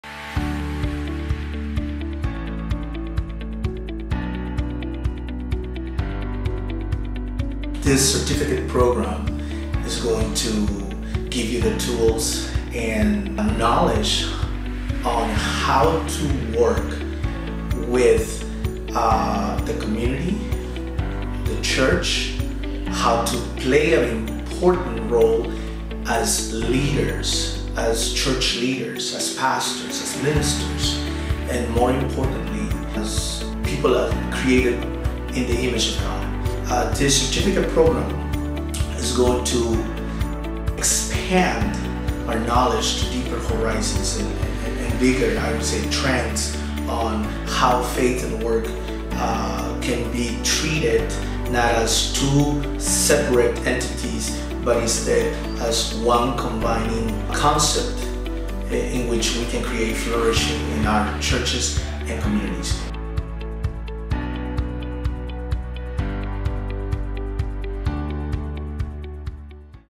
ExCEL Promo 2017
Certificate Program Objectives:
The program is designed to equip pastors, church leaders, and church members with information, knowledge, and awareness in relation to how our Faith guides our work and how that Faith-driven work impacts our economy. The courses will discuss the importance of mature discipleship, biblical stewardship, value creation, productivity, social justice issues and responsible action.

Faith, Work, and Economics Quotient (FWE-Q Assessment)
At the onset of the program, students will be provided a short assessment to measure their level of awareness and understanding of Faith, Work, and Economics.  The student may also be given this assessment at the completion of the program to compare baseline and formative results.